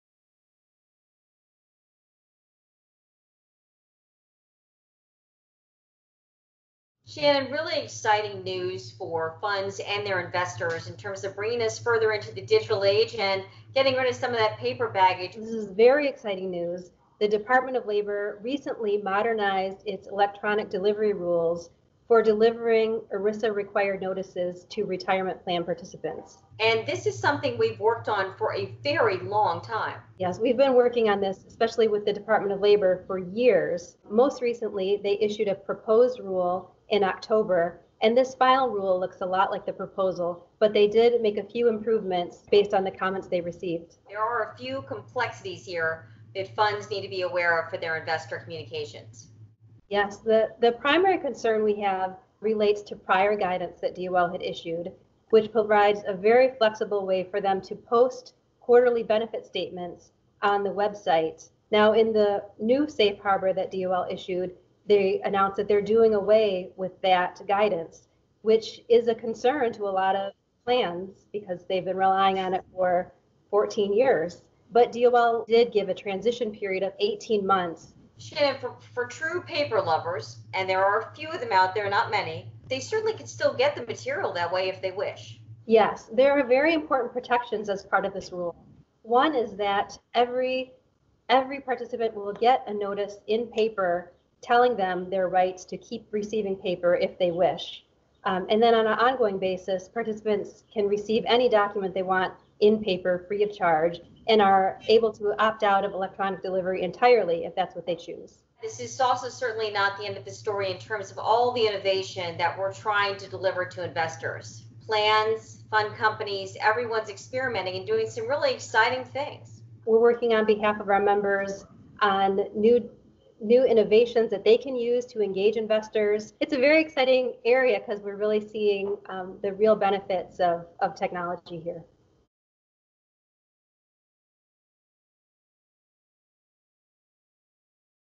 Digital Trend Continues to Transform Investor Communications
New rules governing electronic delivery of 401(k) disclosures are now final. In the June 26, 2020, edition of Focus on Funds, ICI Assistant General Counsel Shannon Salinas describes the latest regulatory developments and broader efforts to modernize how funds engage with their shareholders.

Stephanie Ortbals-Tibbs, ICI director of media relations: Shannon, really exciting news for funds and their investors in terms of bringing this further into the digital age and getting rid of some of that paper baggage.

Shannon Salinas, ICI assistant general counsel for retirement policy: This is very exciting news. The Department of Labor [DOL] recently modernized its electronic delivery rules for delivering ERISA-required notices to retirement plan participants.

Ortbals-Tibbs: And this is something we’ve worked on for a very long time.

Salinas: Yes. We’ve been working on this—especially with the Department of Labor—for years. Most recently, they issued a proposed rule in October, and this final rule looks a lot like the proposal. But they did make a few improvements based on the comments they received.

Ortbals-Tibbs: There are a few complexities here that funds need to be aware of in their investor communications.

Salinas: Yes. The primary concern we have relates to prior guidance that DOL had issued, which provides a very flexible way for them to post quarterly benefits statements on the website. Now, in the new safe harbor that DOL issued, they announced that they’re doing away with that guidance, which is a concern to a lot of plans because they’ve been relying on it for 14 years. But DOL did give a transition period of 18 months.

Ortbals-Tibbs: Shannon, for true paper lovers—and there are a few of them out there, not many—they certainly can still get the material that way if they wish.

Salinas: Yes. There are very important protections as part of this rule. One is that every participant will get a notice, in paper, telling them their rights to keep receiving paper if they wish. And then on an ongoing basis, participants can receive any document they want in paper, free of charge, and are able to opt out of electronic delivery entirely if that’s what they choose.

Ortbals-Tibbs: This is also certainly not the end of the story in terms of all the innovation that we’re trying to deliver to investors. Plans, fund companies—everyone’s experimenting and doing some really exciting things.

Salinas: We’re working on behalf of our members on new innovations that they can use to engage investors. It’s a very exciting area because we’re really seeing the real benefits of technology here.